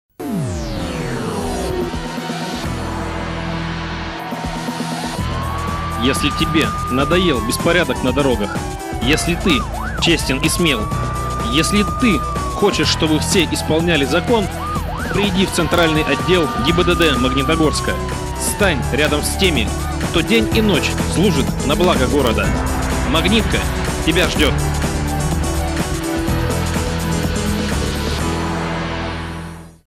to serve & to protect.flv
Челейкин - имиджевый ролик службы в ГАИ - to serve & to protect .